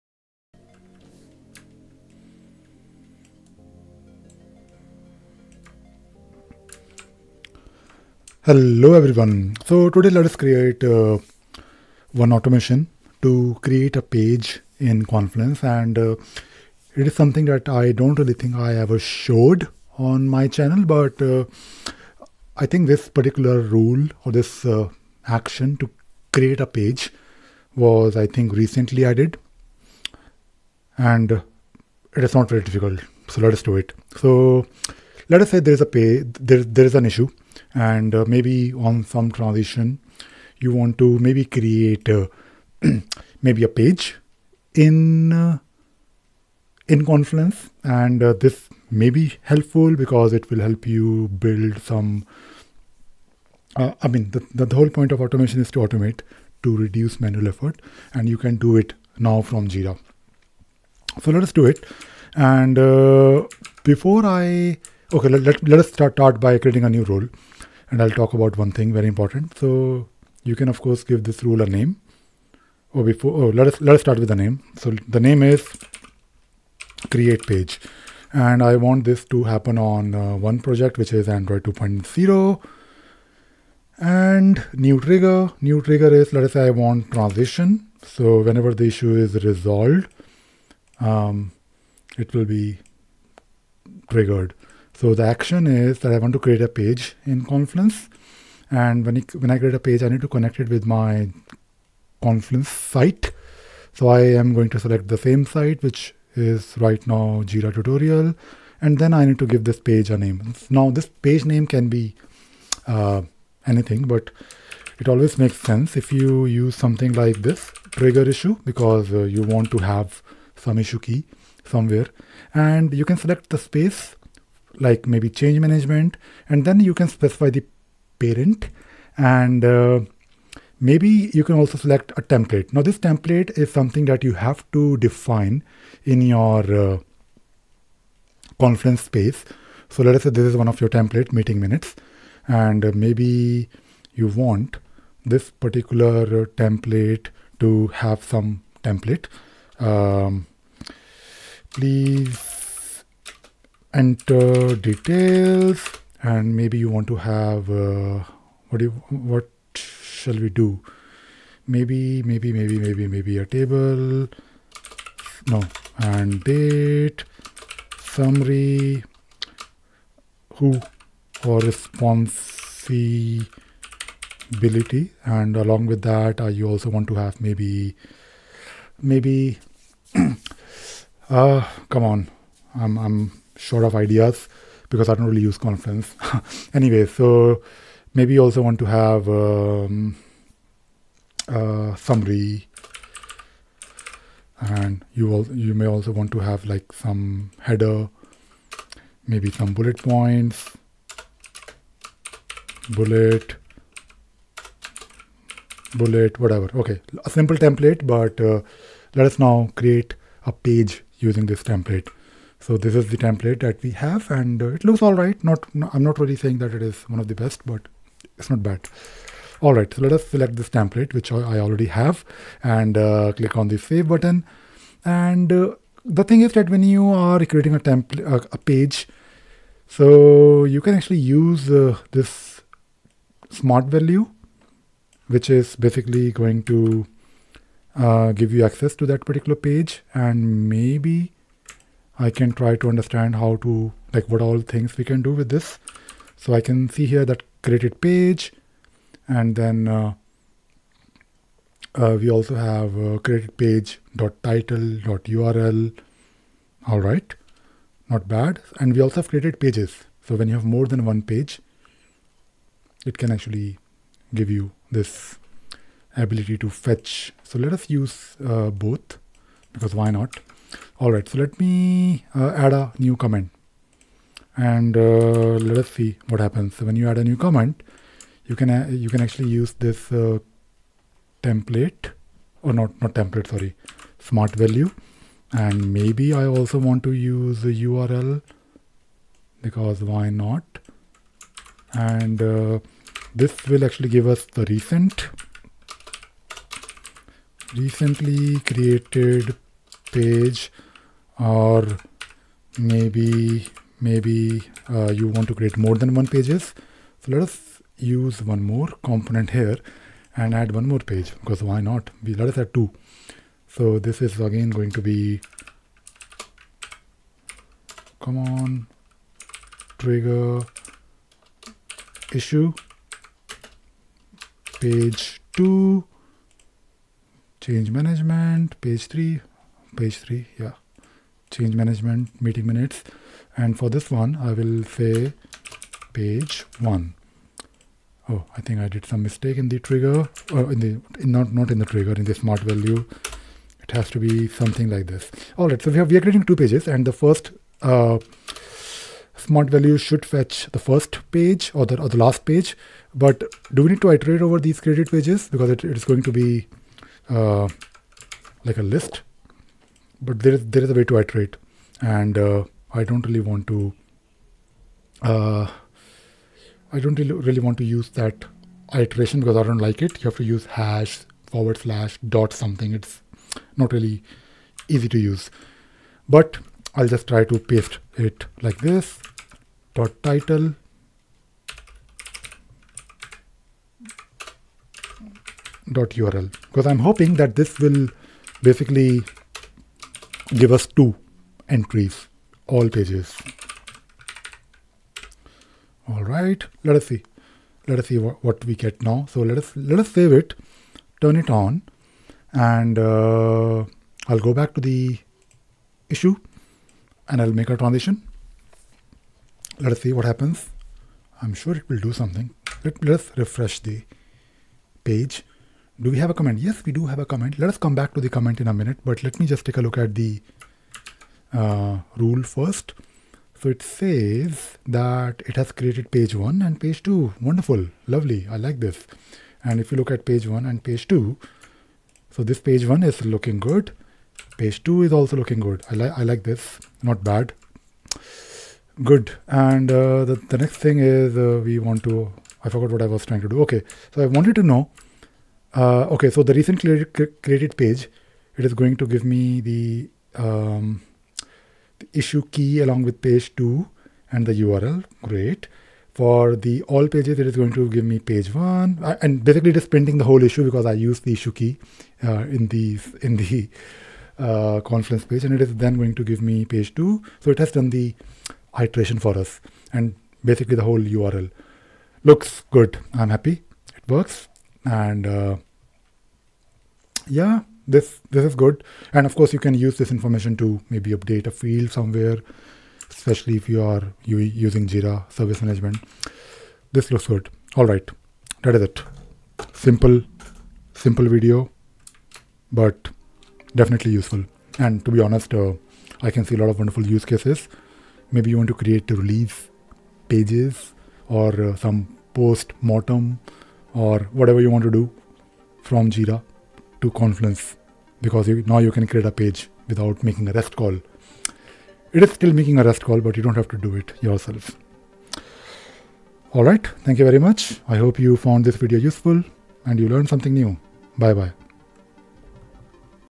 Jira Cloud Automation - Create page in confluence
You can easily create a Confluence page from Jira using a simple automation.